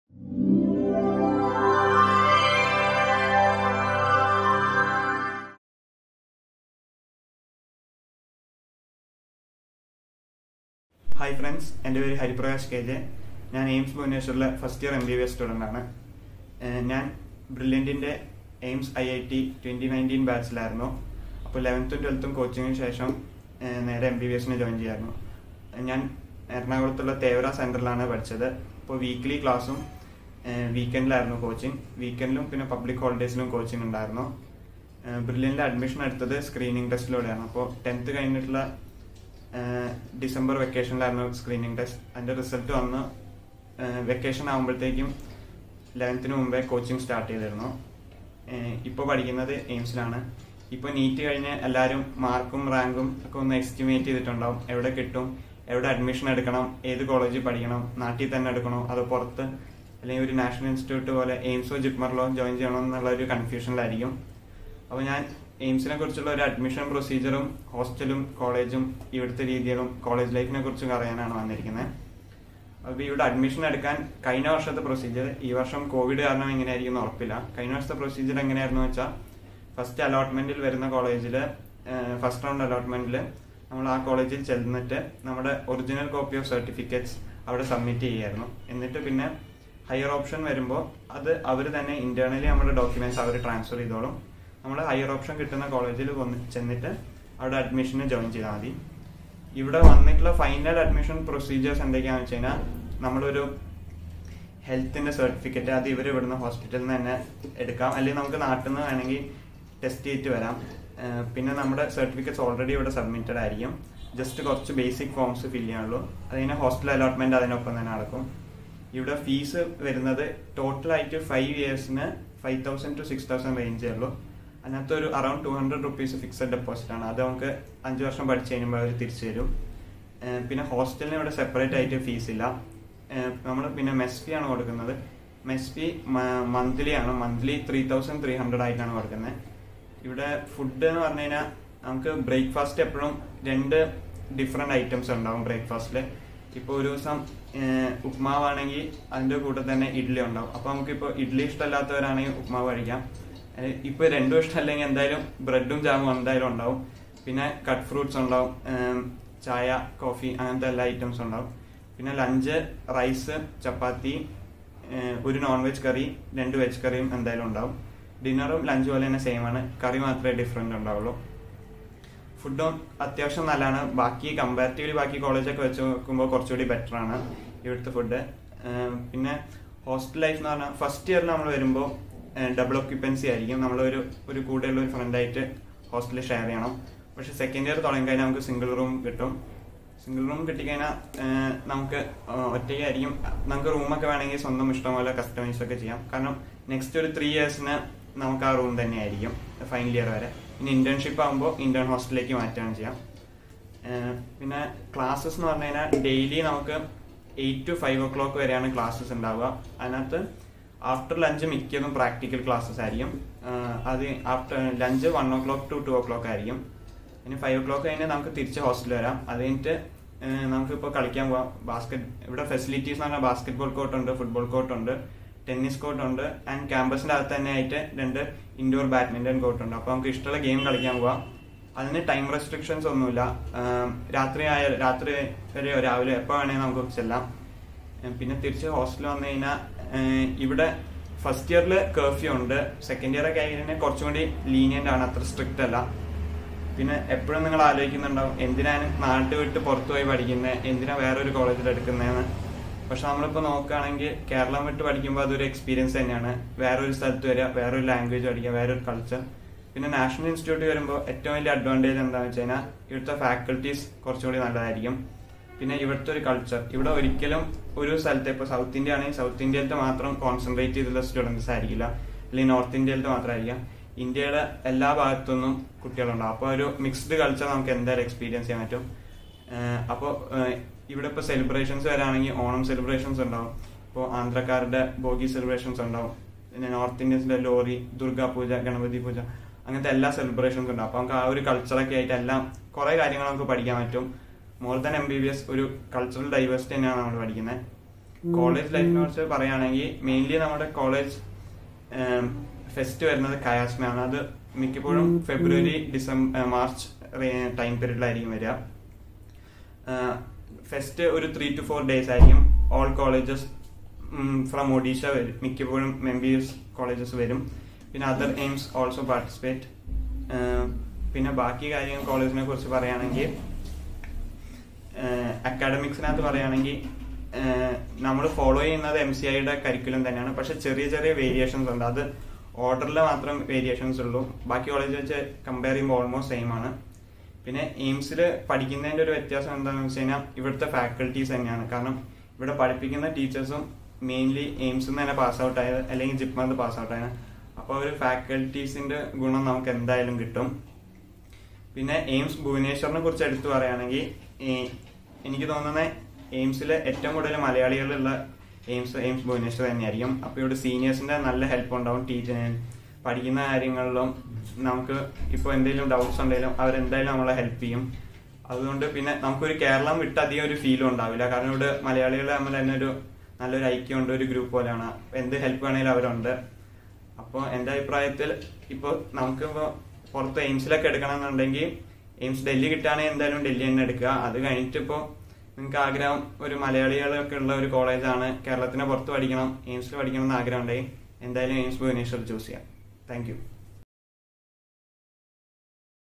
AIIMS അറിയേണ്ടതെല്ലാം | Admission Procedure | Hostel | College Life | Hariprakash K J
Hariprakash K J
AIIMS Bhubaneswar
First Year MBBS Student
Brilliant 2019 Batch